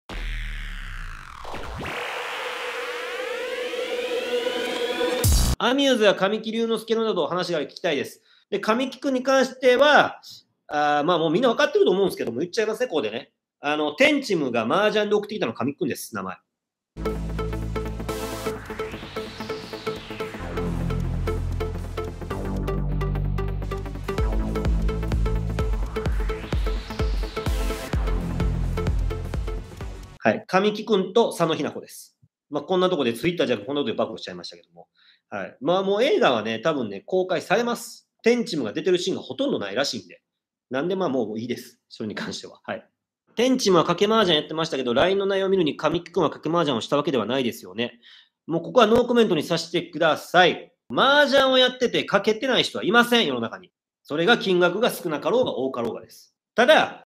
【流れ弾】えっ！てんちむ麻雀賭博の新メンバー？♡神木隆之介は映画主演やけど大丈夫かな？♡俳優♡グラビア♡【東谷義和/切り抜き/暴露/闇】
全ての闇に裁きを！同志よ集え！
ガーシー様が芸能界に天変地異を起こす姿を目に焼き付けろ！